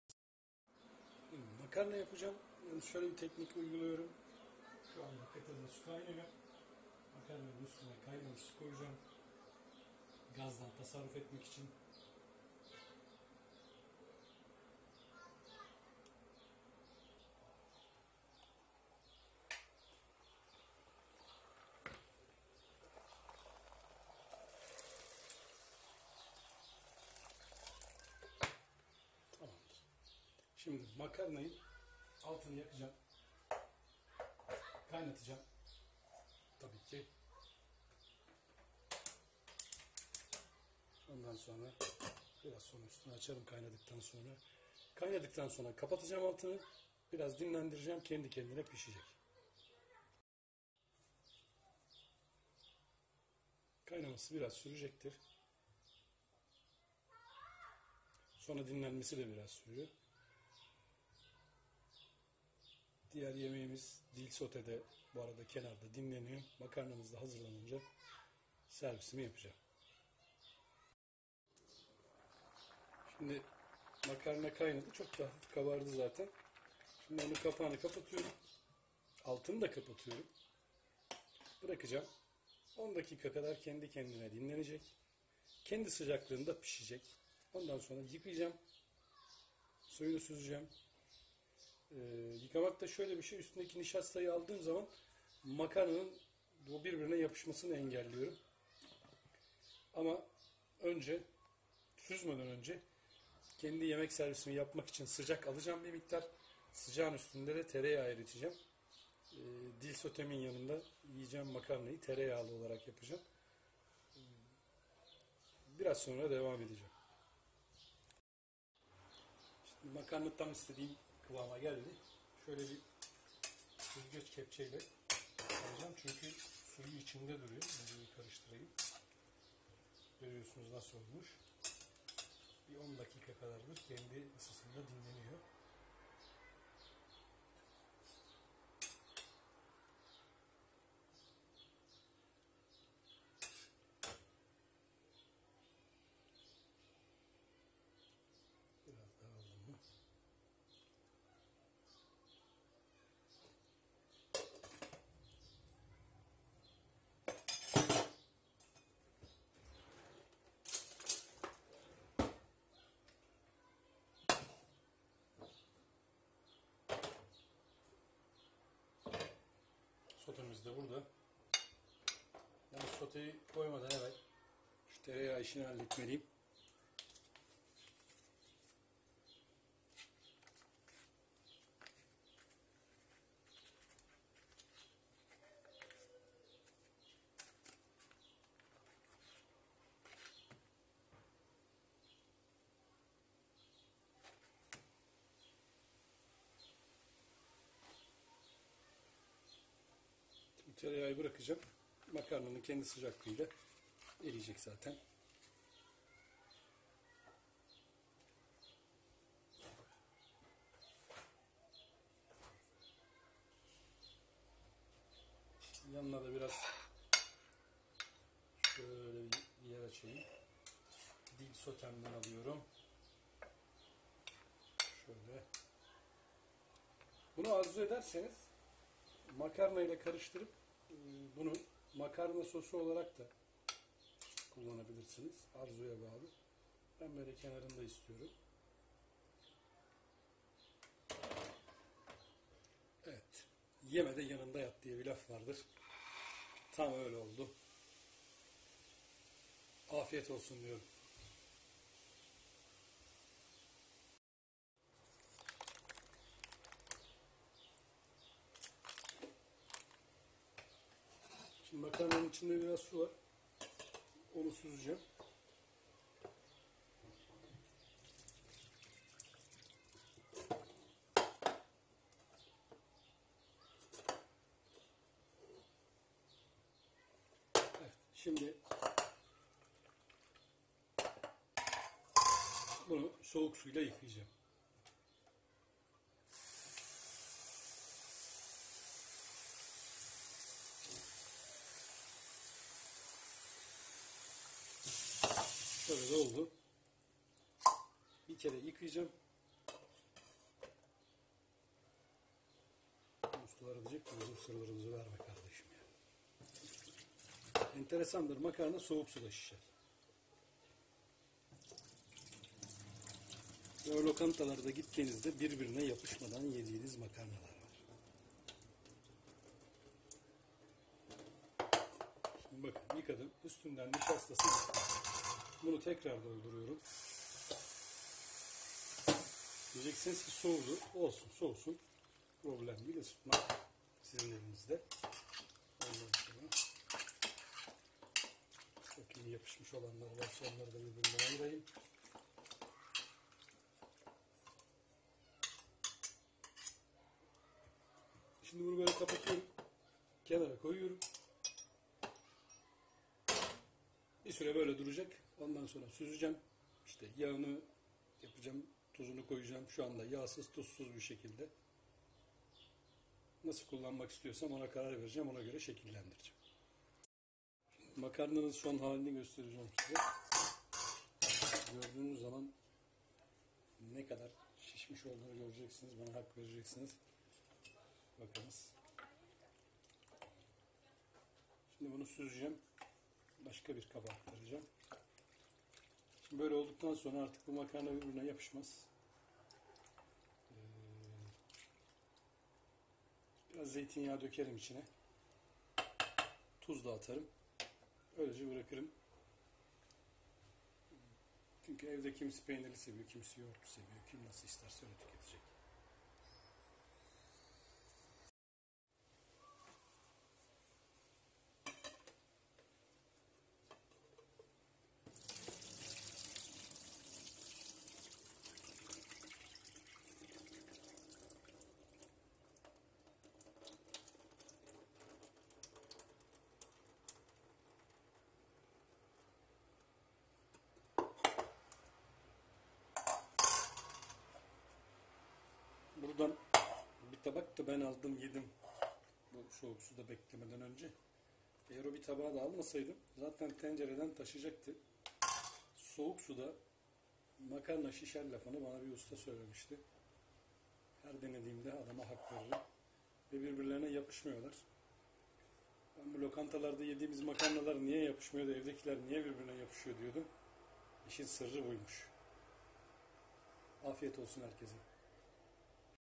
Usta İşi Yapışmayan Makarna Ustalardan Alınmış Makarna Tüyoları
arada bir profosyonelleri dinlemek iyi oluyor
1 Paket Makarna
su
tuz
yağ